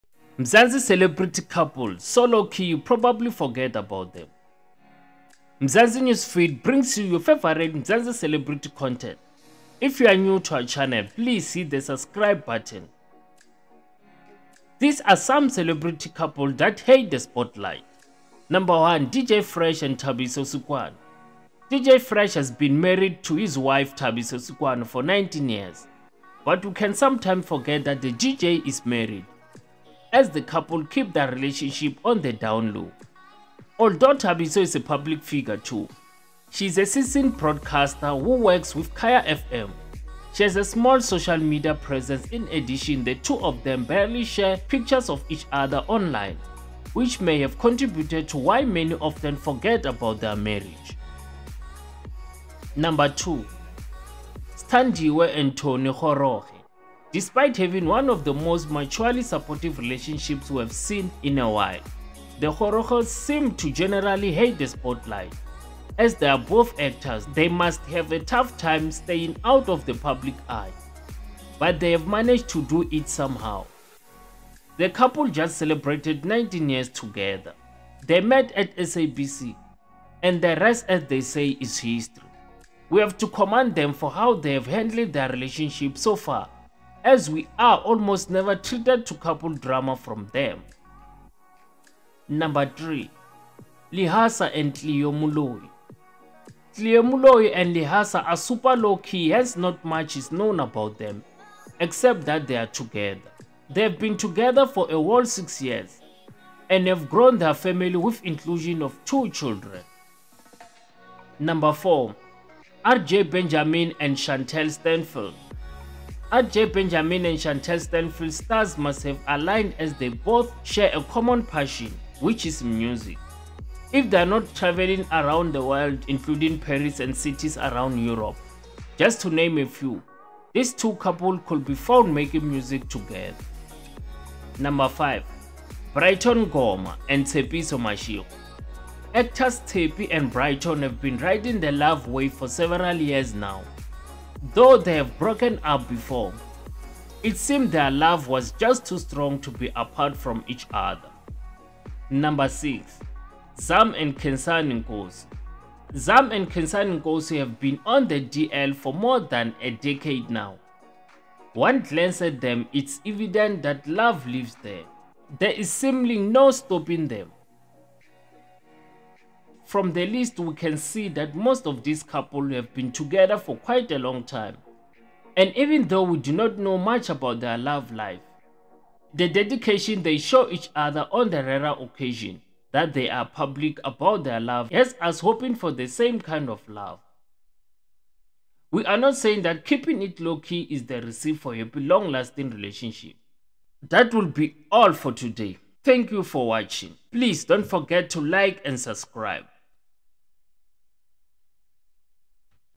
6 South African Celebrity Couples Who HATE The Spotlight
We look at South African celebrity couples who are married and are so low key that people don't know they are together the likes of sthandiwe and tony kgoroge